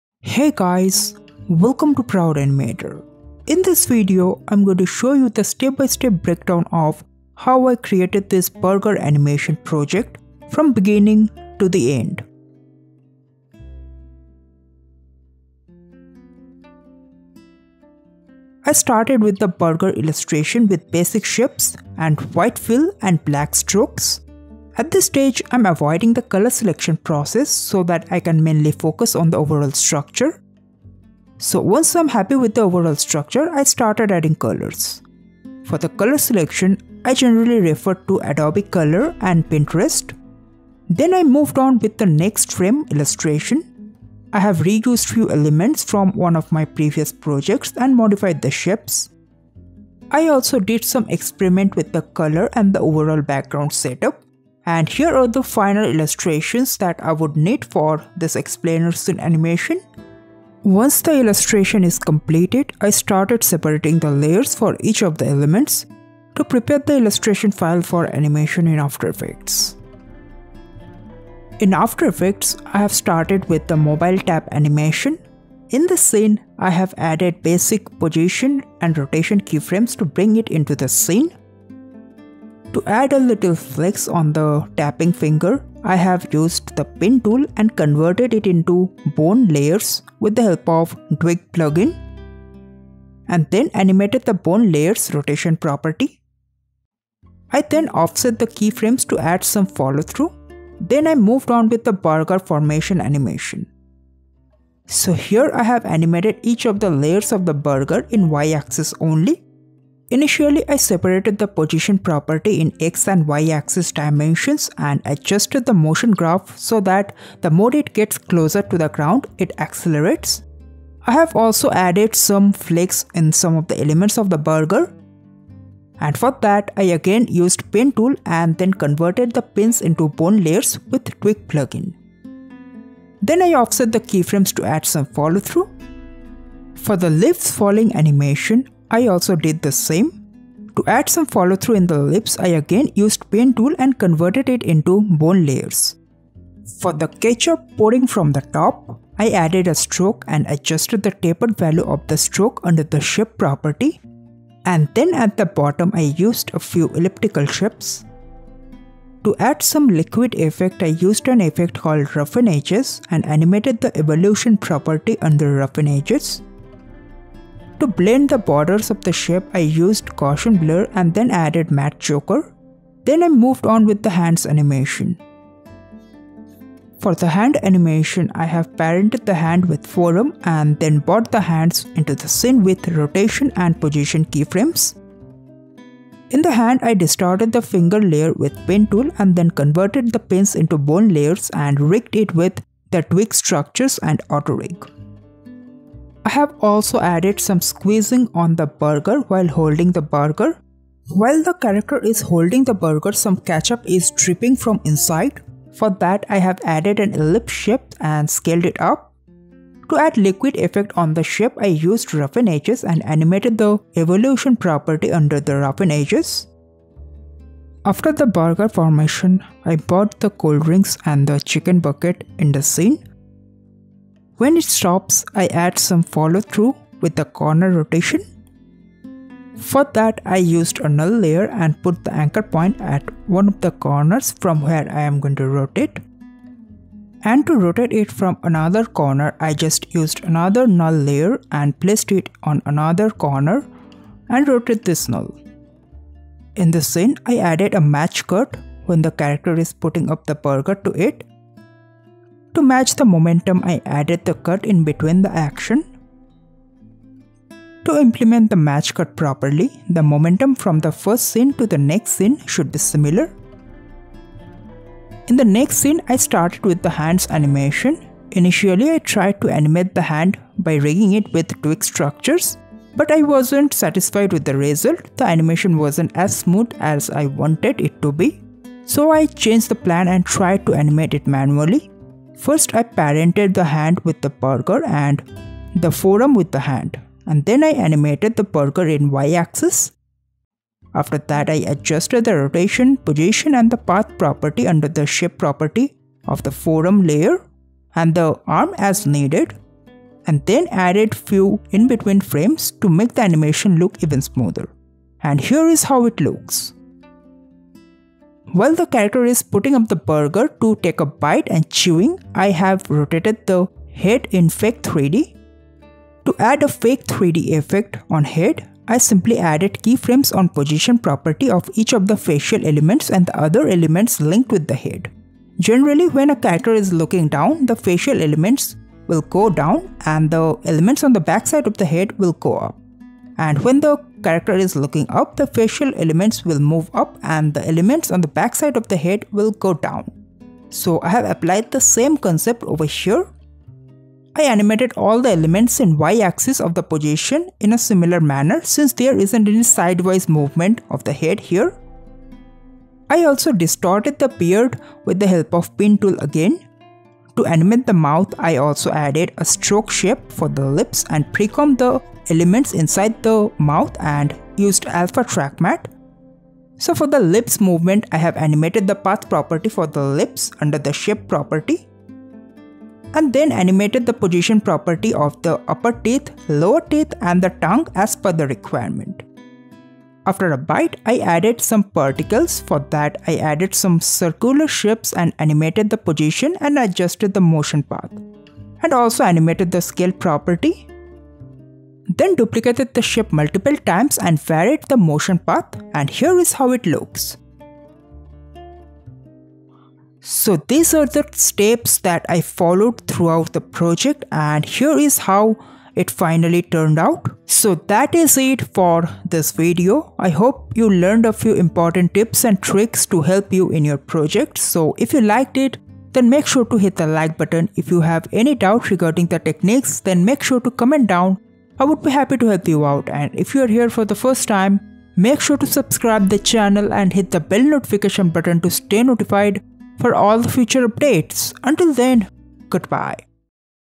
Burger Animation Project Breakdown || After Effects
Hey Guys,
Welcome to Proud Animator.
In this video, I am going to show you guys the step-by-step breakdown of how I created the burger animation project.